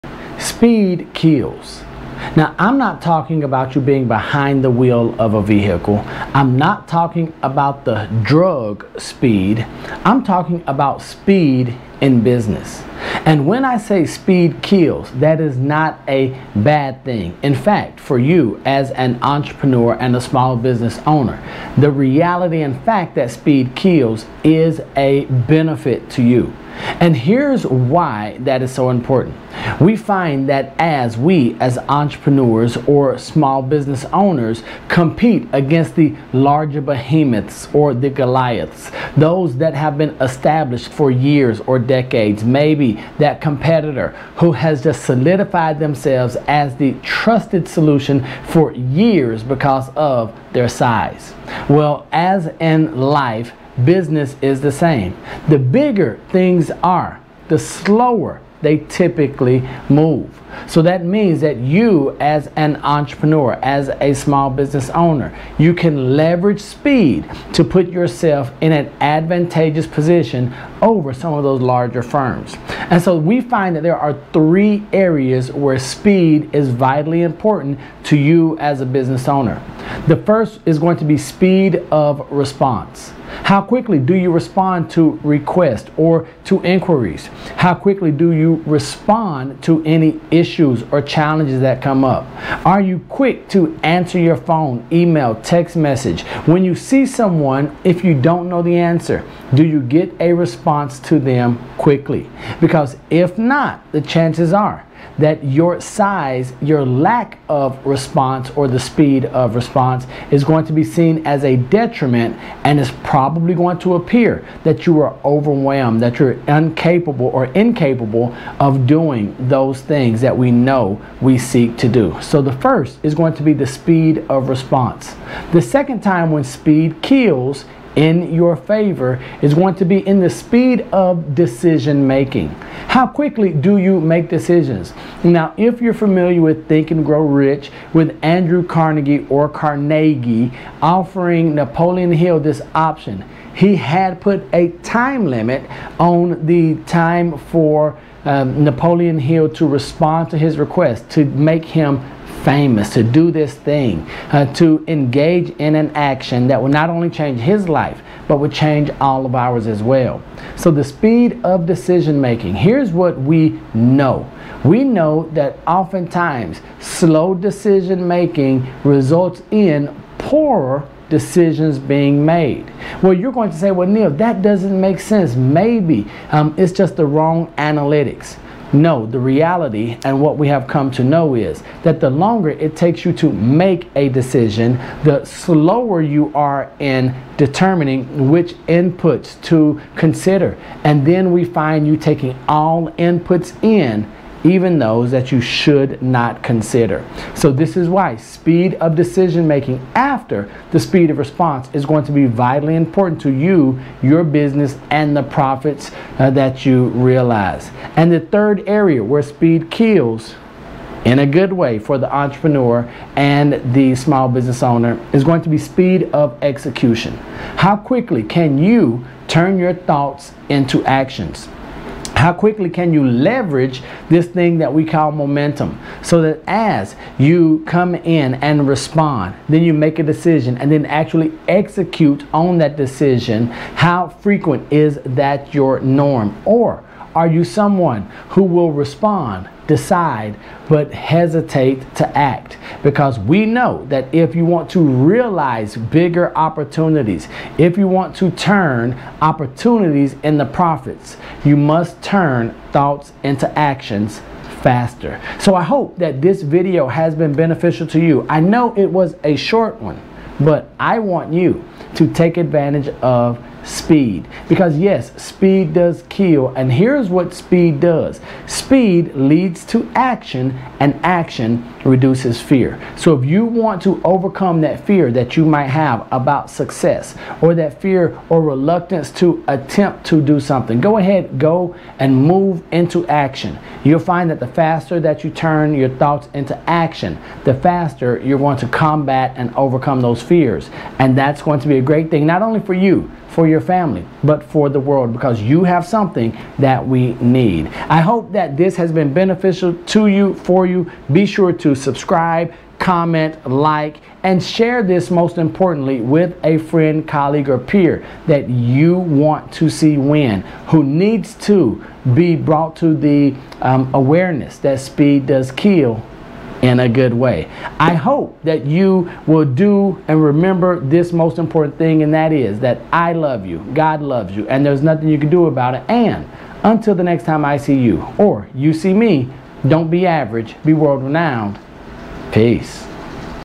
Crush the Competition & Your Goals
Speed Kills... and saves at the same time! Are you the victim or benefactor?
There are few things that small business owners and entrepreneurs can do as important as leveraging speed.
Embracing speed in three key areas makes all the difference and can propel you to getting your unfair share of profit.
Here's how you can crush the competition and your goals at the same time.

Speed Matters:
- Speed of Response
- Speed of Decision Making
- Speed of Execution

Don't be average... Be World Renowned!